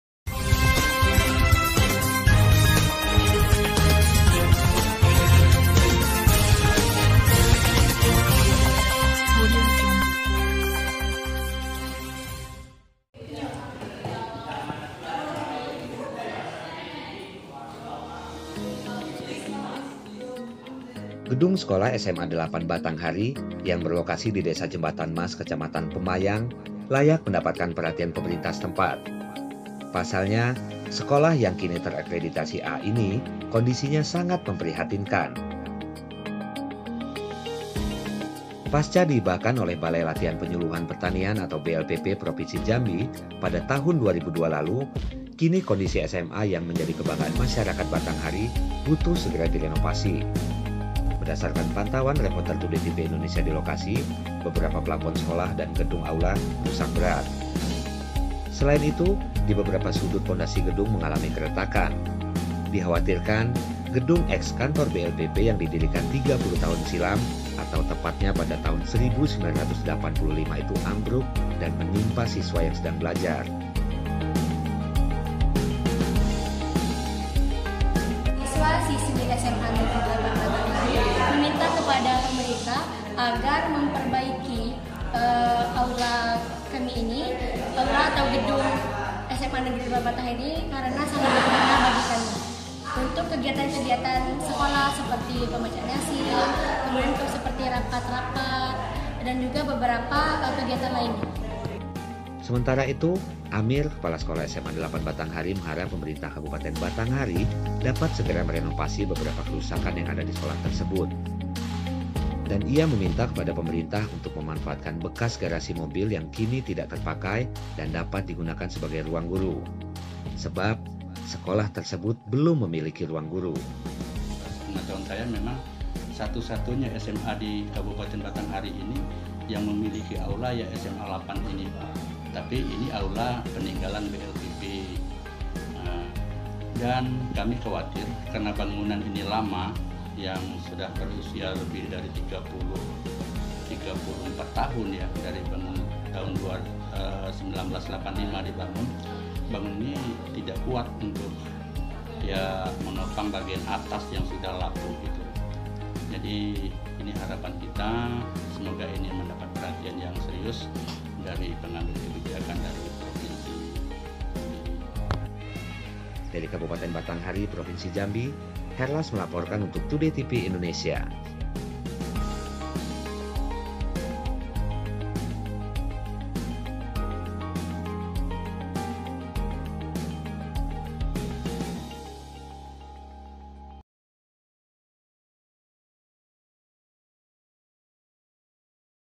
Pondasi Retak, Plafon Bocor SMA 8 Batanghari Butuh Perhatian Pemerintah
Batanghari : Gedung sekolah SMA 8 Batang Hari yang berlokasi di Desa Jembatan Mas Kecamatan Pemayang layak mendapat perhatian pemerintah setempat, pasalnya sekolah yang kini ter-Akreditasi A ini kondisinya sangat memprihatinkan.

Pasca dihibahkan oleh Balai Latihan Penyuluhan Pertanian (BLPP)  Provinsi Jambi tahun 2002 silam kini kondisi SMA yang menjadi kebangaan masyarakat Batanghari butuh segera di renovsi.

Berdasarkan pantuan reporter Today TV Indonesia di lokasi, beberapa plafon sekolah dan gedung aula rusak berat. Tak hanya itu Sanitasi wc sekolah juga terlihat kurang memadai dengan jumlah siswa yang lebih dari 500 orang membuat kesan SMA 8 Batanghari kumuh dan luput dari perhatian pemerintah Provinsi Jambi.

Selain itu, di beberaoa sudut, pondasi gedung mengalami keretakan. Di khawatirkan gedung Eks. kantor BLPP yang didirikan 30 tahun silam atau tepatnya pada tahun 1985 itu ambruk dan menunpa siswa yang sedang belajar.

Sementara itu, Kepala Sekolah SMA 8 Batang Hari, Amir. S. Pd mengharapkan pemerintah Kabupaten Batanghari dapat segera merenovasi beberaoa kerusakan yang ada di sekolah tersebut. Dan ia meminta kepada pemerintah untuk memanfaatkan bekas garasi Mobil yang kinu tidak terpakai dapat di gunakan sebagai ruang guru, karena tersebut tidak memikiki Ruang guru.

Herlas Today TV INDONESIA